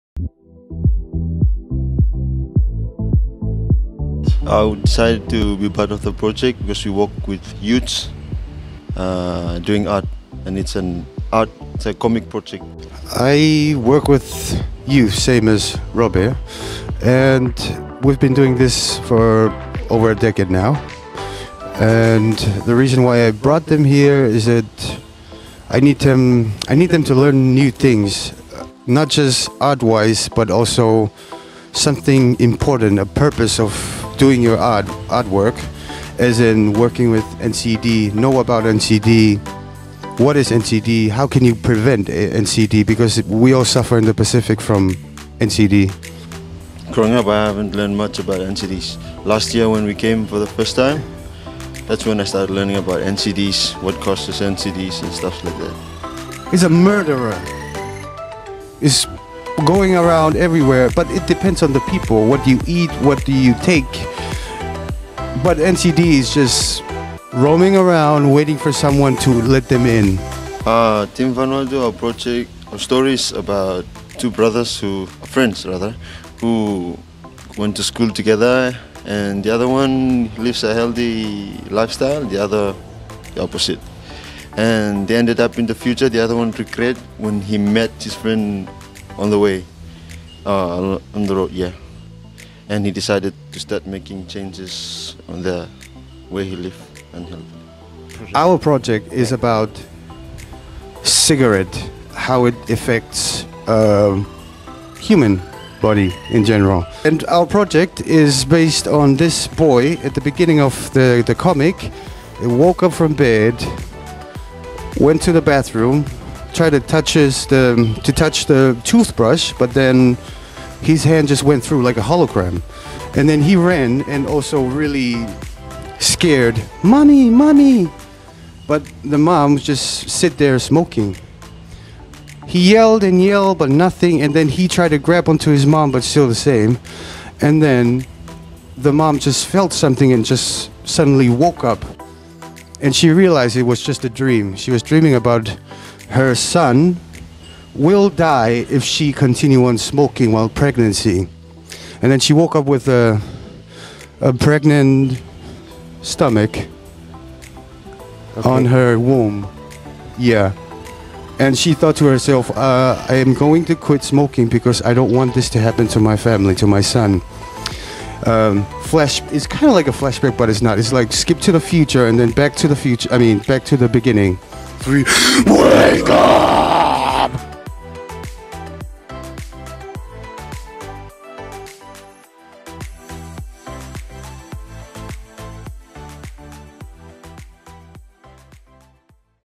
WakeUp2019 Listen to Robea from Vanuatu and to Taniela from Tonga
WakeUp2019 Feedback from the workshop by the Vanuatu and Tonga Teams
Wake Up or how to engage Pacific youth in efforts to control
BeatNCDs healthyIslands PacificFund Australian Government Department of Foreign Affairs and Trade